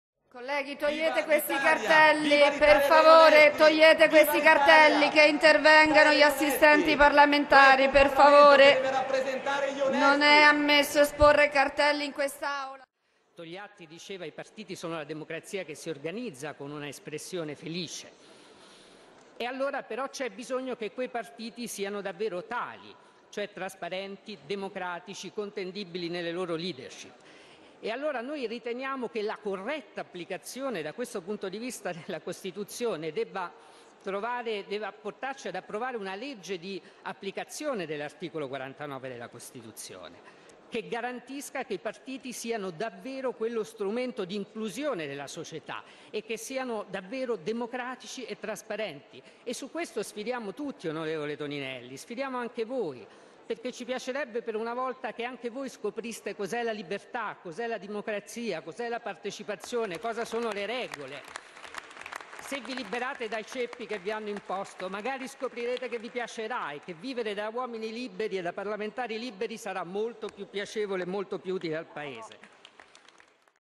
Cyborg Orfini sfotte Toninelli : provate libertà e democrazia , vi piaceranno
Danilo Toninelli M5S , Matteo Orfini PD